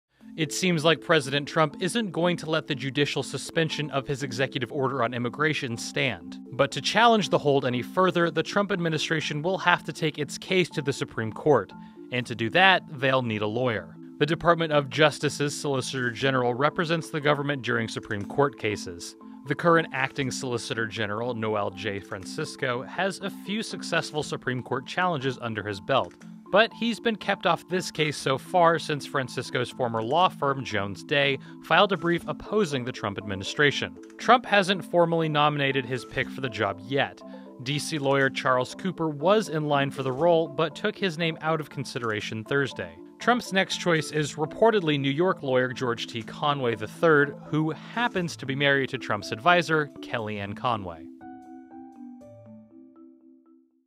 Trump needs a solicitor general
Trump was reportedly considering Charles Cooper for the role — until Cooper rejected the prospect.